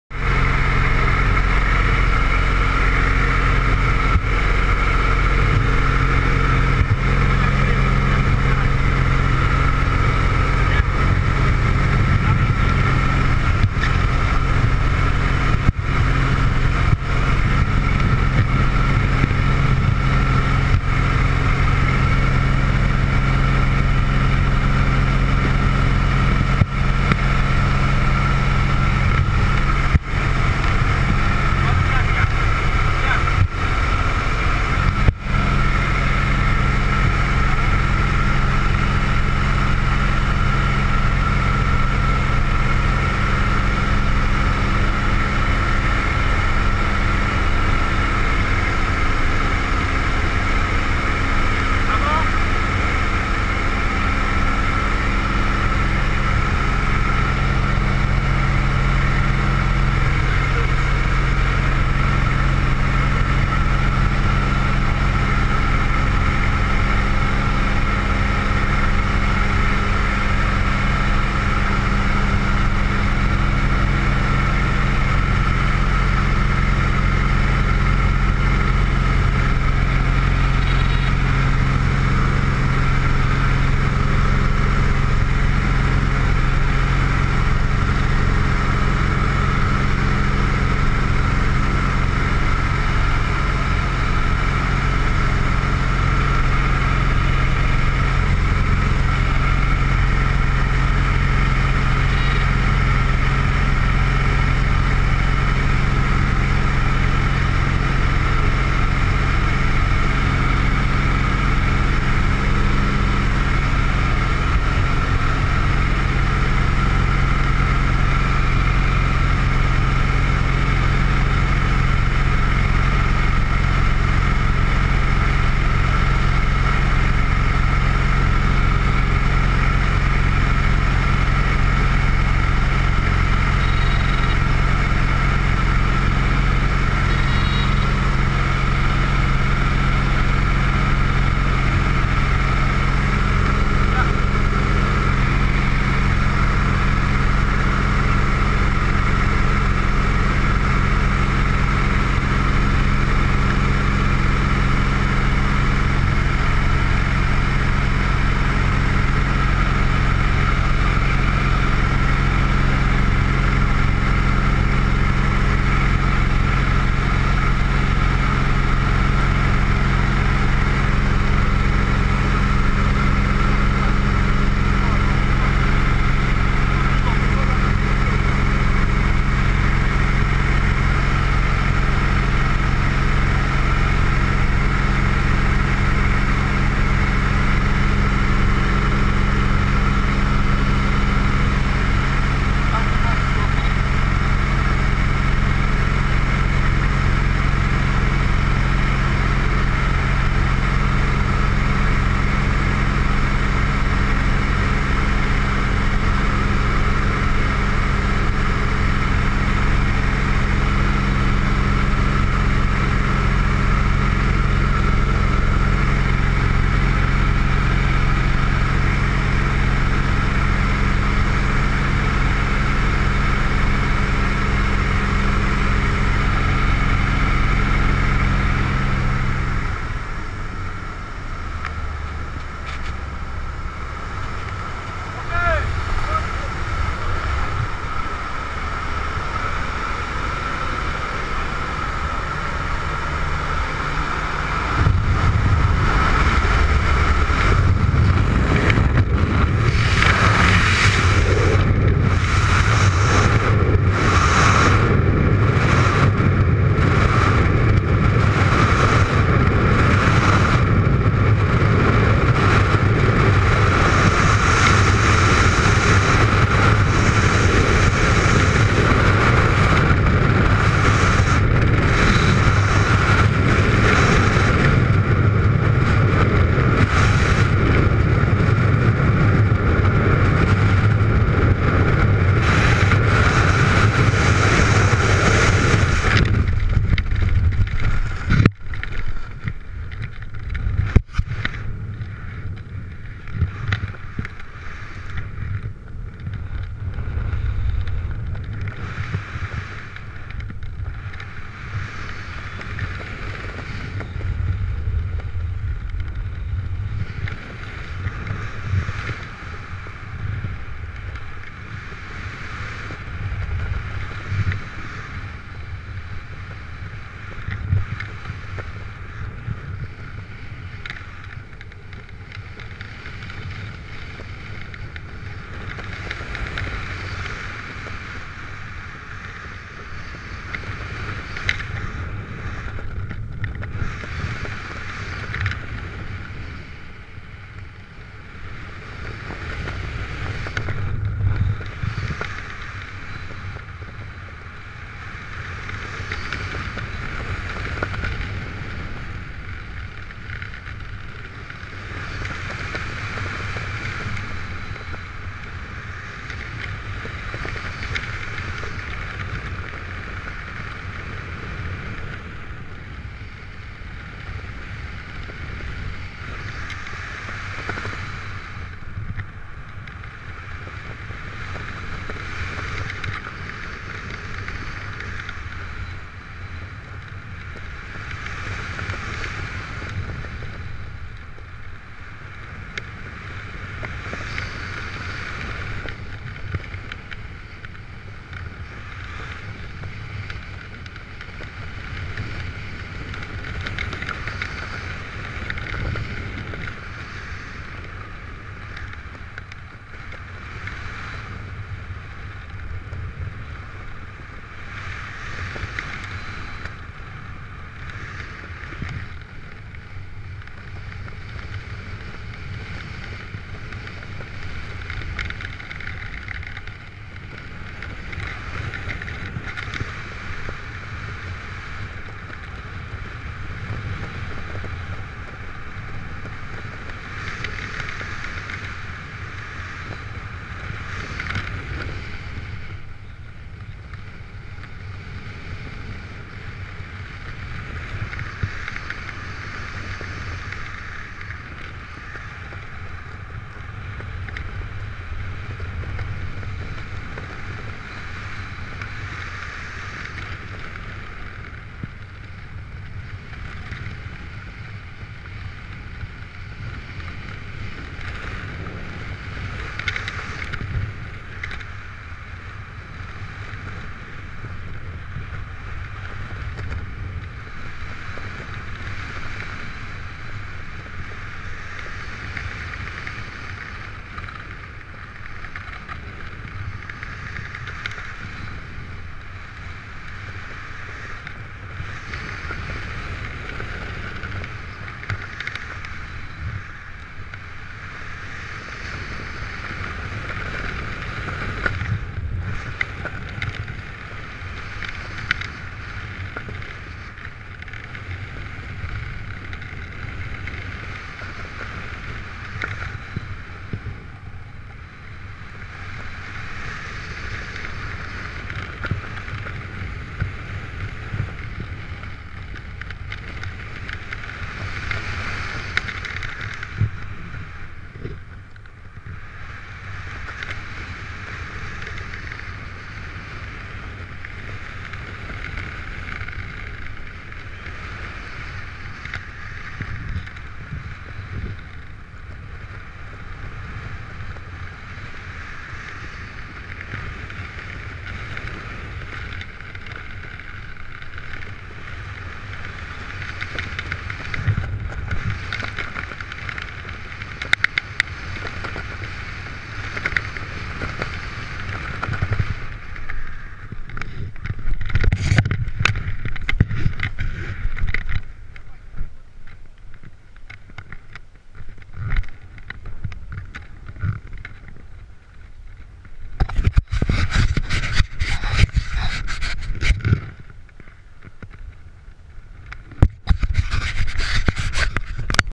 Salto em Tancos, Vitor Martins no "Para Clube os Boinas Verdes".
Queda livre em Tancos, Asas ao Peito.